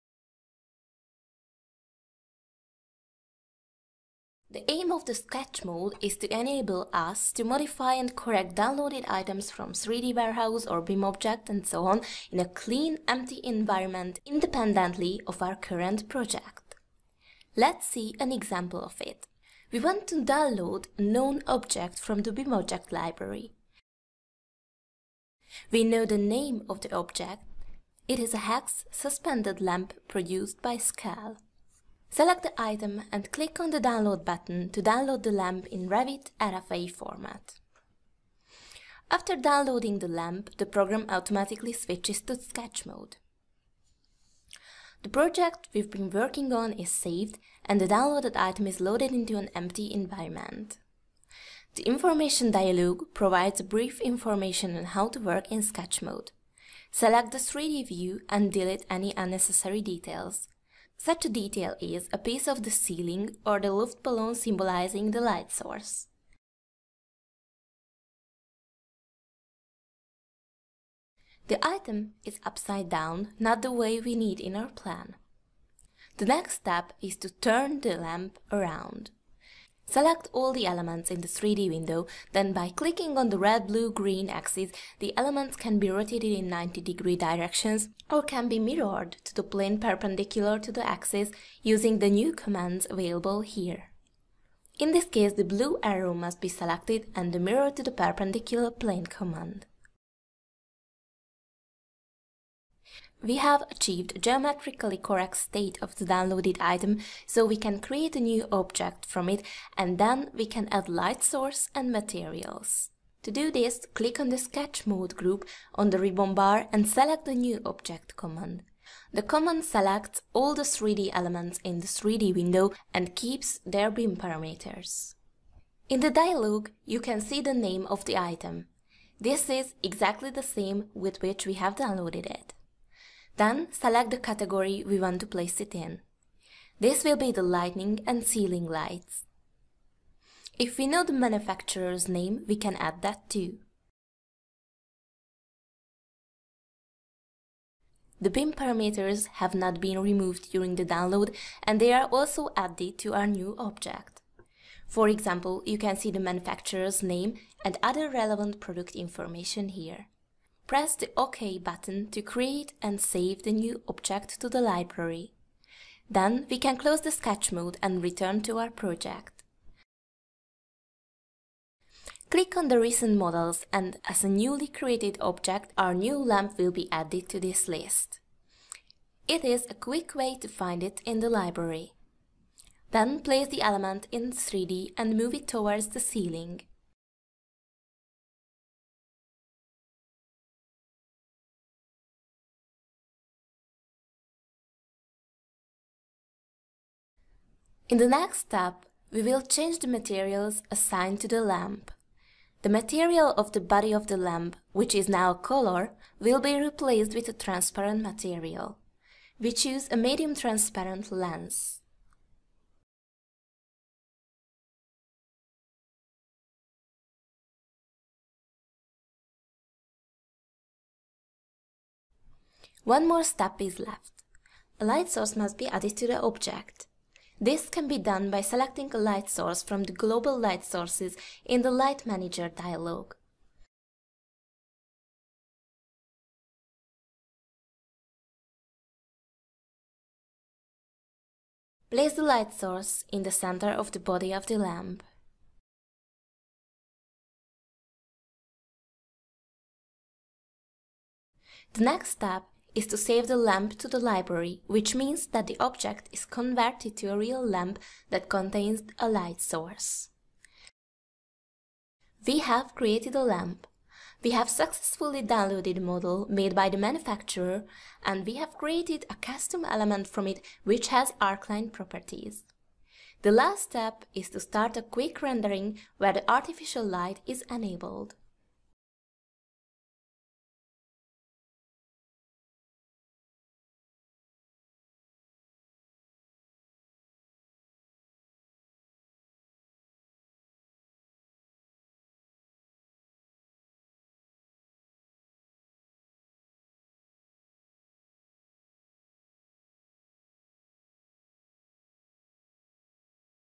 [ARCHLine.XP] Getting Started - Sketch Mode
Sketch mode is a new feature in ARCHLine.XP 2018. It enables you to edit and geometrically modify objects downloaded from different object libraries (e.g. : BIMobject, 3D Warehouse etc...) in a clean and empty environment independently from your current project.